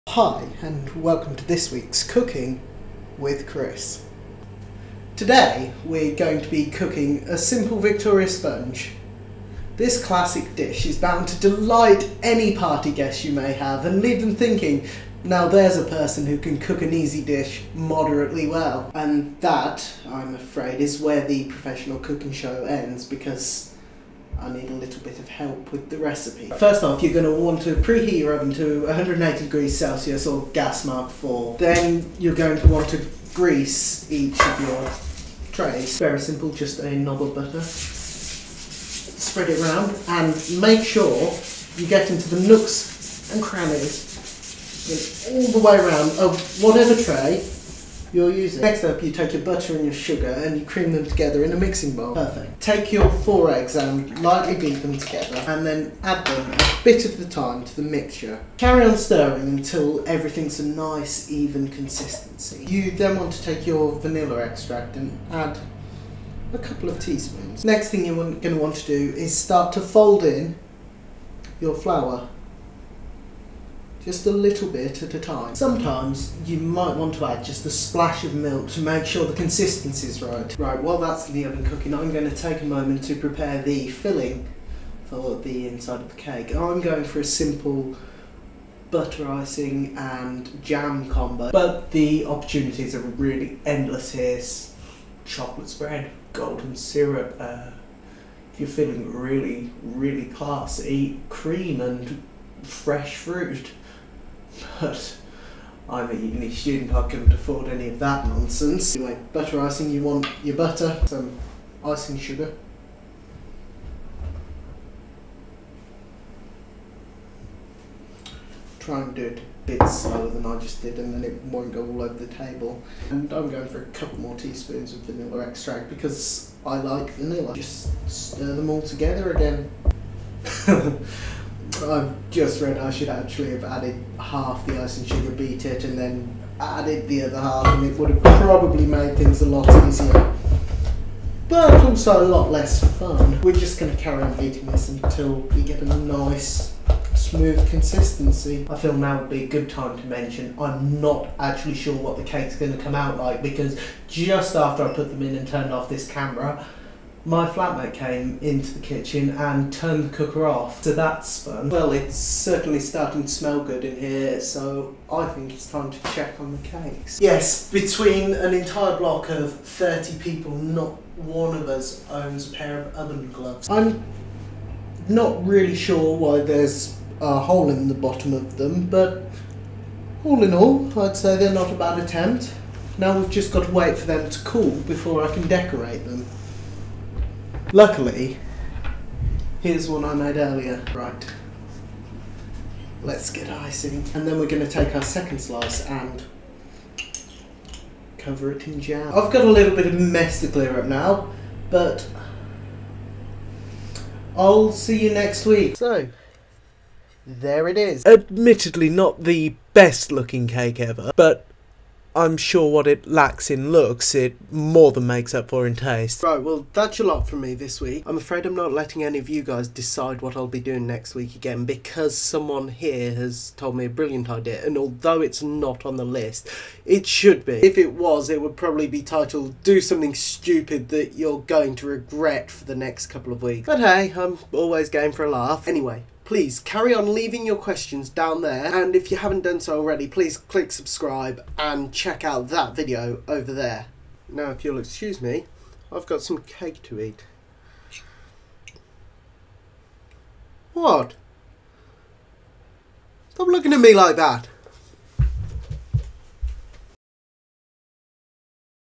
Cooking with Chris - New Years Revelations
Sorry this took so long!  I've been busy, but that's no excuse so my block mates have set me the cinnamon challenge as a punishment.  The video will hopefully be up before the next video!

Anyway, here's me making a cake.  Enjoy!